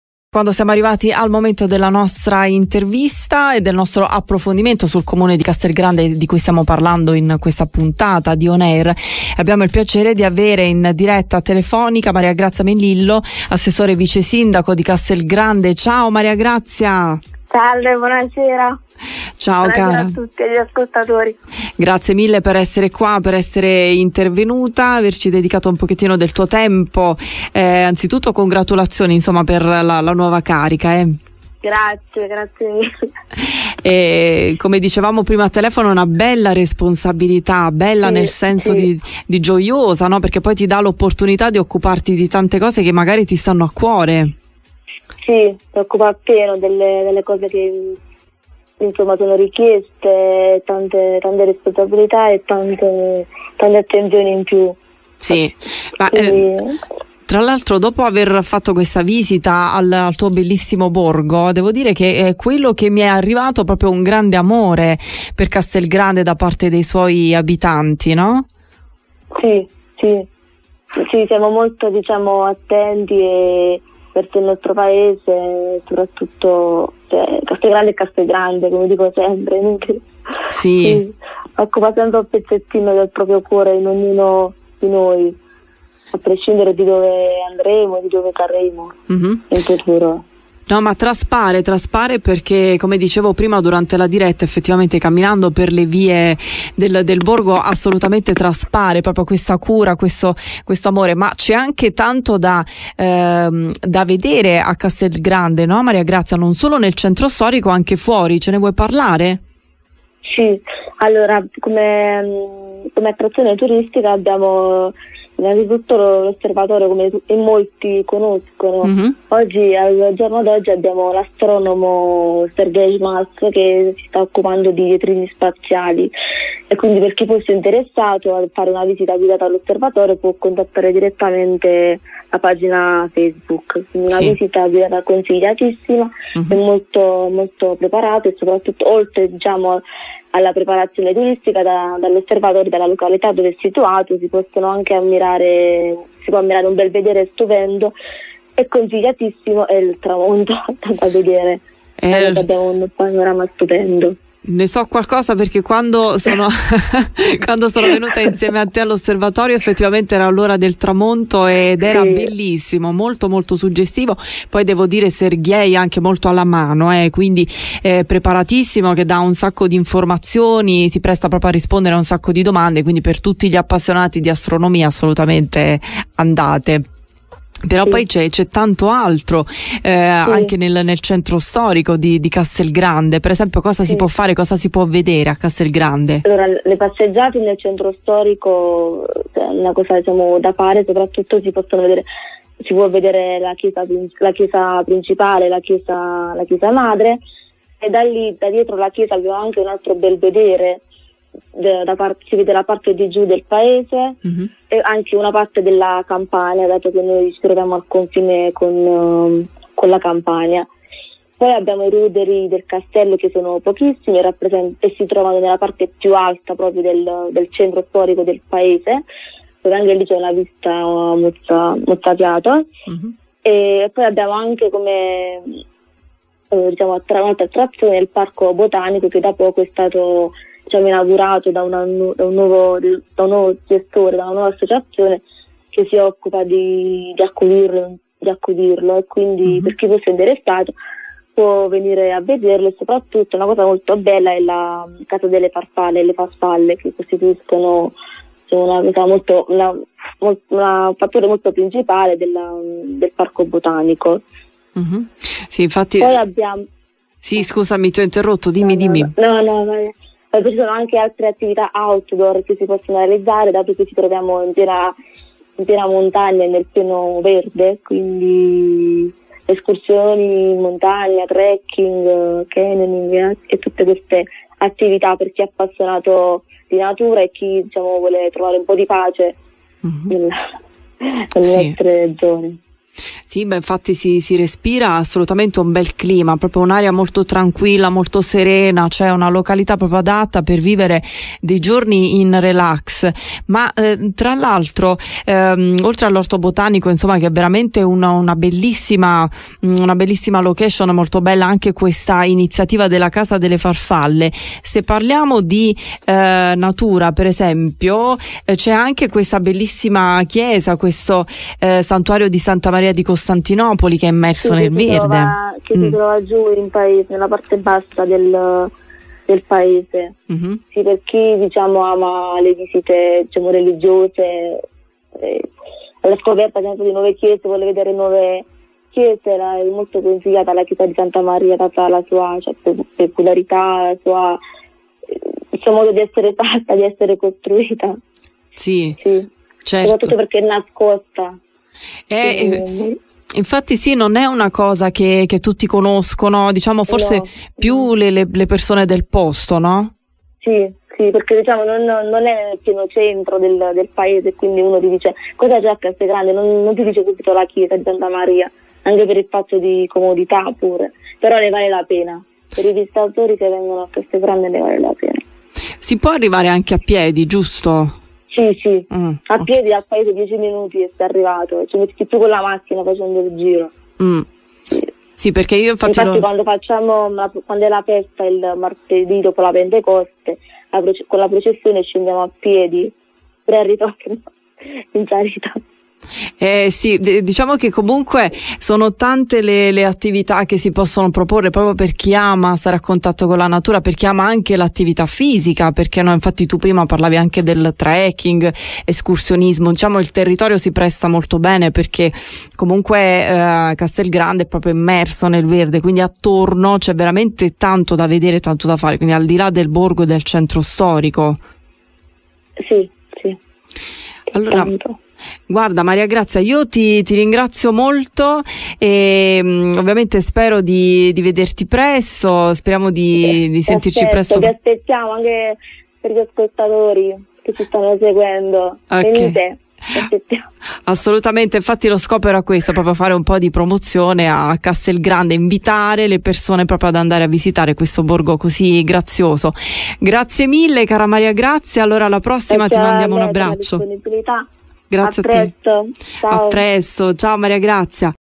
Speciale Borghi del Sud Italia - Castelgrande (Pz) Intervista con Mariagrazia Melillo
Speciale Borghi del Sud Italia - Castelgrande (Pz) Intervista con Mariagrazia Melillo, assessore e vicesindaco di Castelgrande